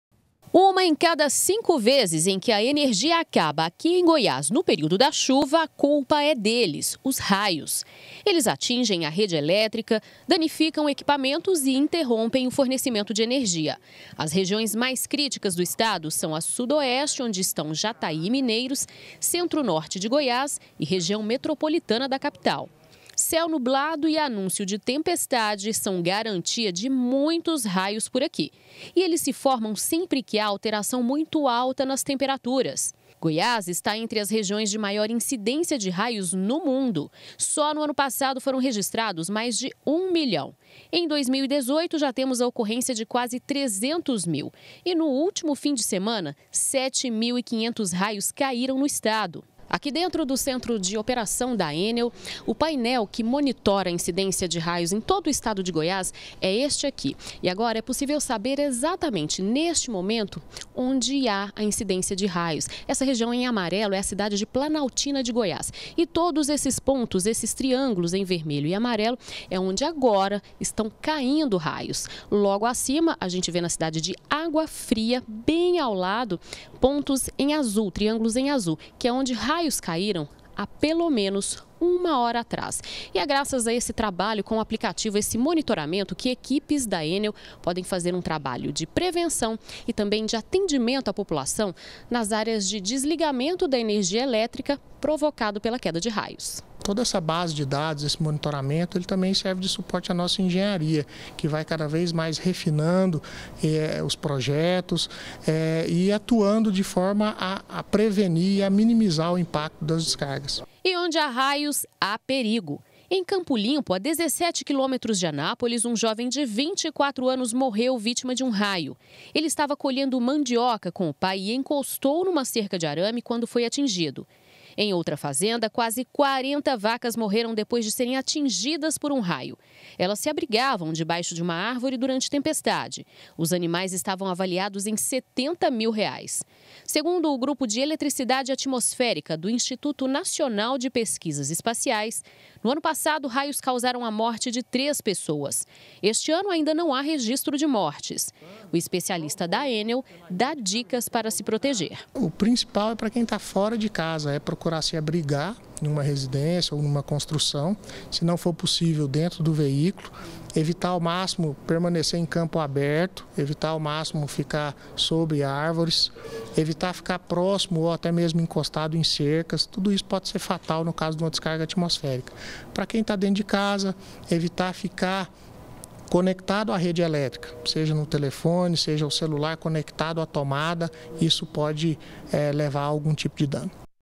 GR - Raios: ENEL monitora em tempo real - 20-03-2018
O estado de Goiás está entre as regiões com maior incidência de raios no país. E a ENEL, antiga CELG, monitora, em tempo real, as condições do clima para agilizar o  atendimento dos locais atingidos por esses raios.